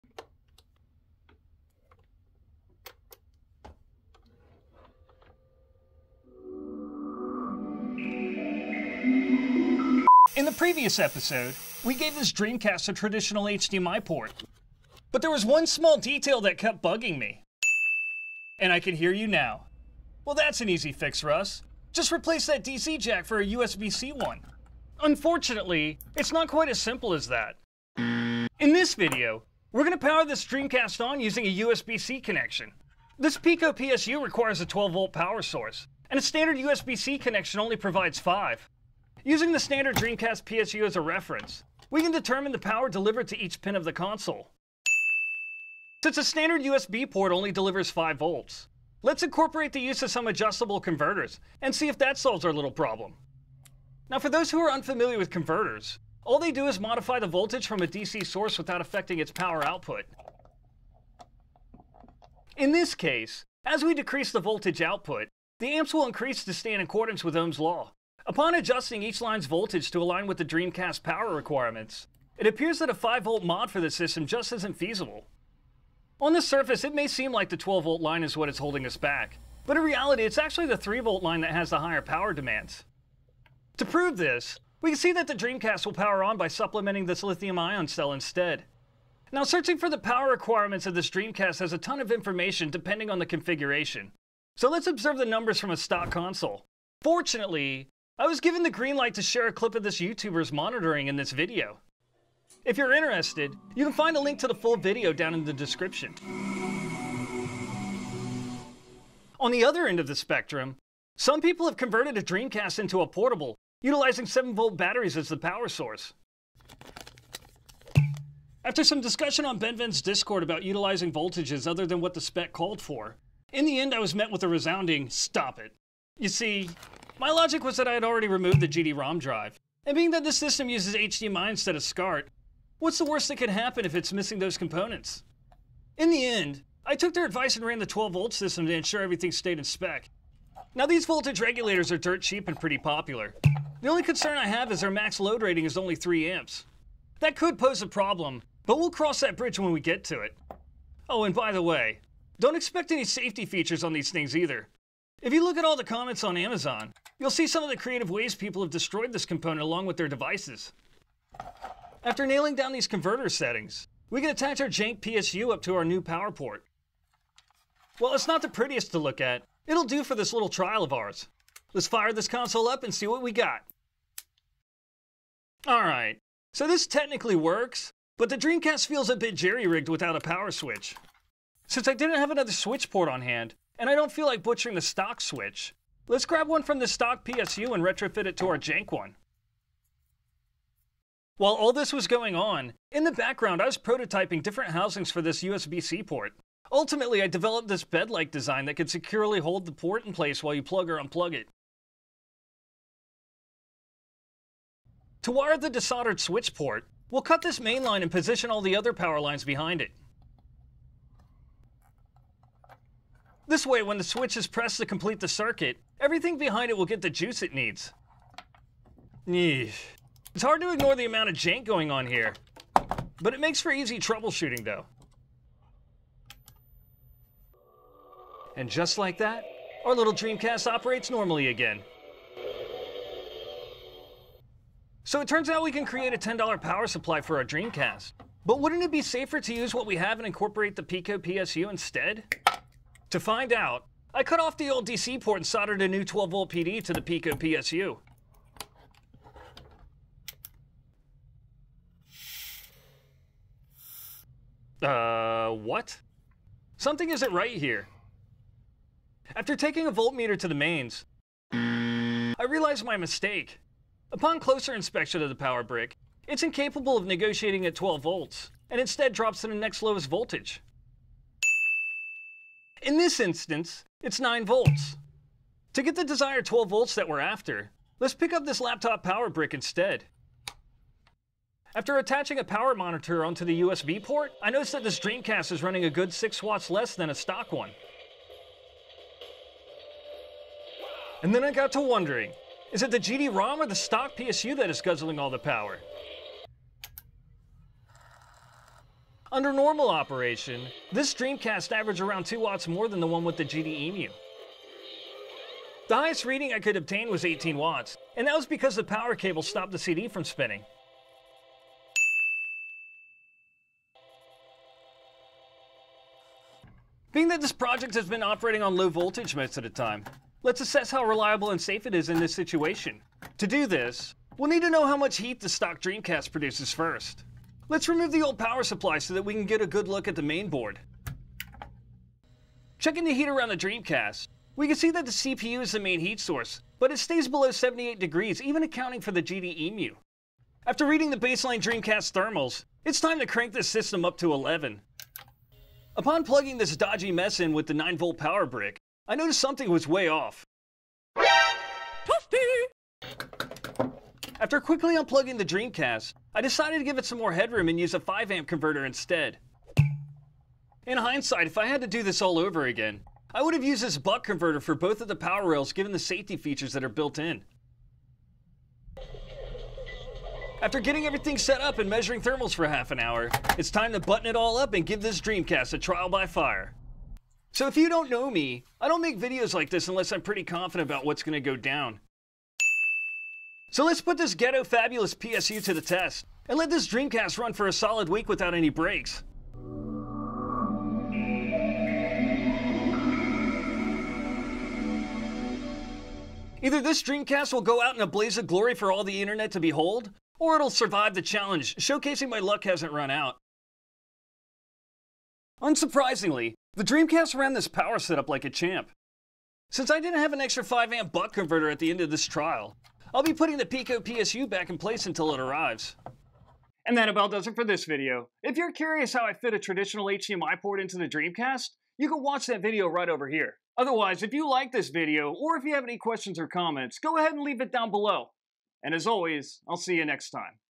Let's Power This Dreamcast With USB-C!
Last time we replaced the serial port of this Dreamcast with a traditional HDMI port, but the power supply's DC port irked me.
Let's remedy that and replace it with a standard USB C port.

Special thanks for Bennvenn and Zarky for their patience in our discussions.

Special thanks to Redhunomt for letting me use a clip of his video found

I do NOT profit from any of your purchase decisions!

Parts used:

XL4015 (preferred)

12-Volt USB C PD

LM2596 (for tinkering and fun)

USB C Power Supply

Thermal Camera

3D Printed USB C Housing (size could be off since i sized the original by 1000% in cura)